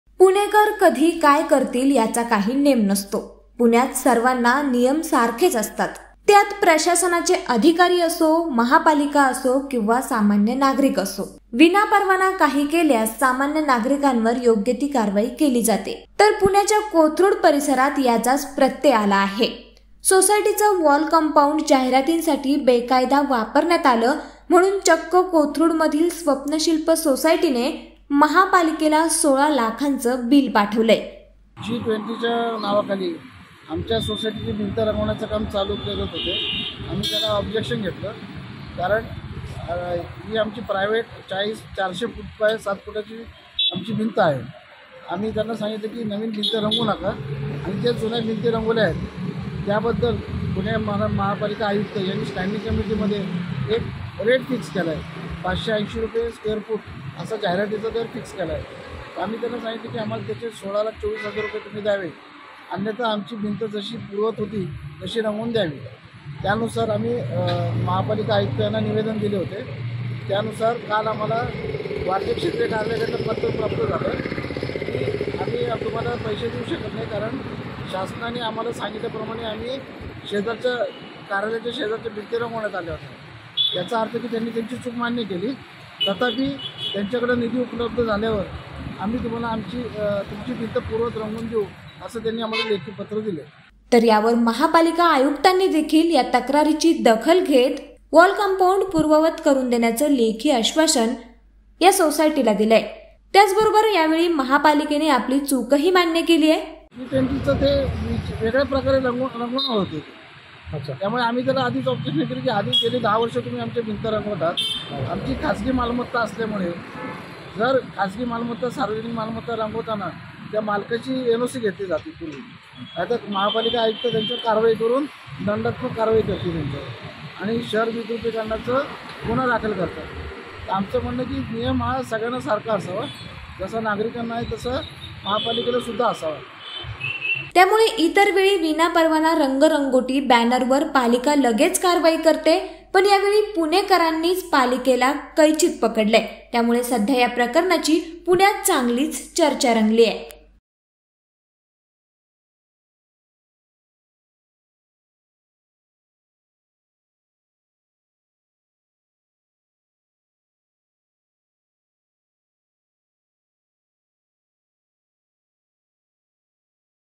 Pune News : सोसायटीची भिंत रंगवली अन् सजग पुणेकरांनी... | LetsUpp Marathi
पुणेकर कधी काय करतील याचा काही नेम नाही. पुण्याच्या कोथरुड परिसरात असाच प्रत्यय आला. काय आहे हे प्रकरण जाणून घेण्यासाठी व्हिडीओ संपूर्ण पाहा...